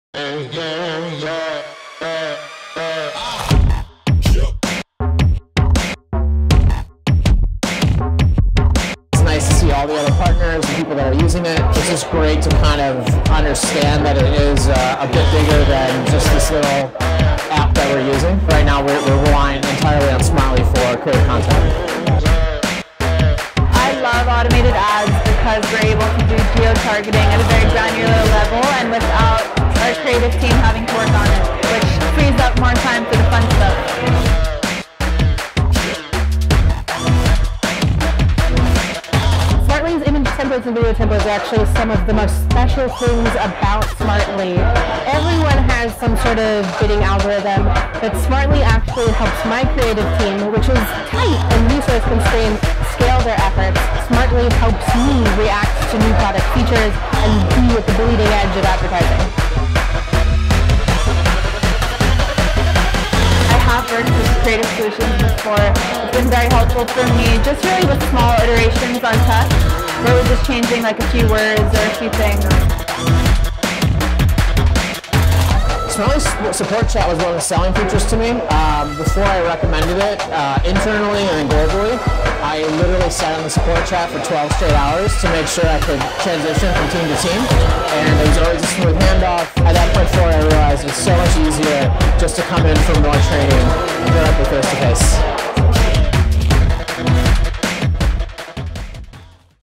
Smartly.io Event - Connect: New York, November (2018)
Smartly.io Connects is a series of pop-up events around the world, created to foster local communities for paid social experts – hence the name ‘Connect.’